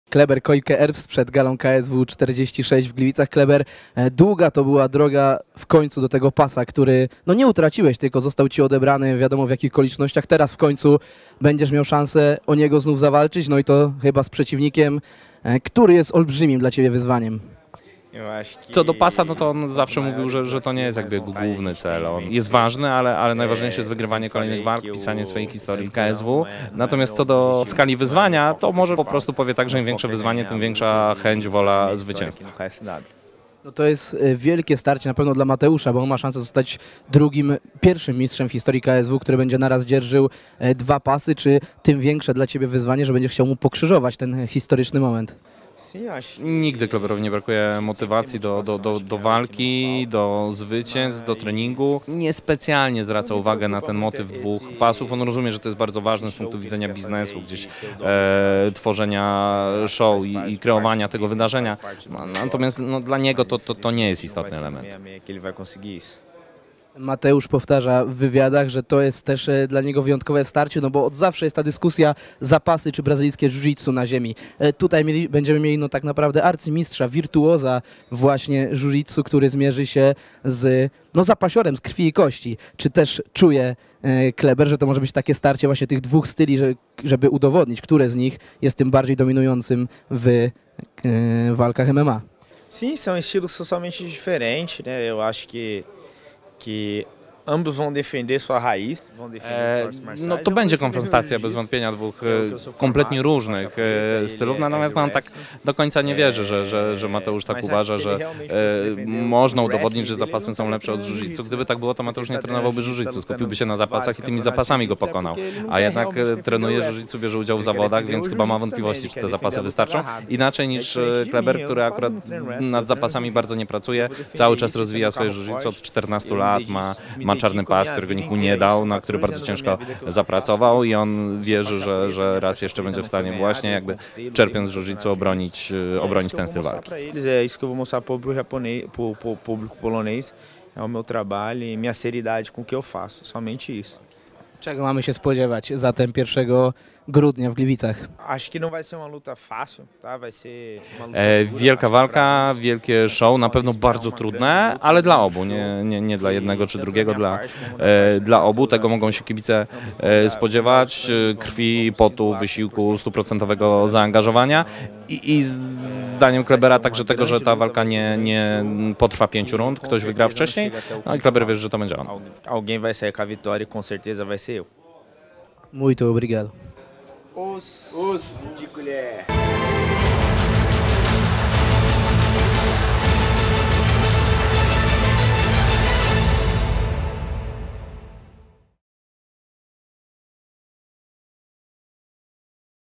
KSW 46: Kleber Koike Erbst - To będzie trudna walka dla obu
Były mistrz KSW w wadze piórkowej Kleber Koike Erbst w krótkiej rozmowie przed starciem z Mateuszem Gamrotem na gali KSW 46 w Gliwicach. Stawką pojedynku będzie pas wagi do 145 funtów.

Bilety na galę KSW 46, w tym te przy wejściu zawodników, już dostępne na w serwisie eBilet.pl: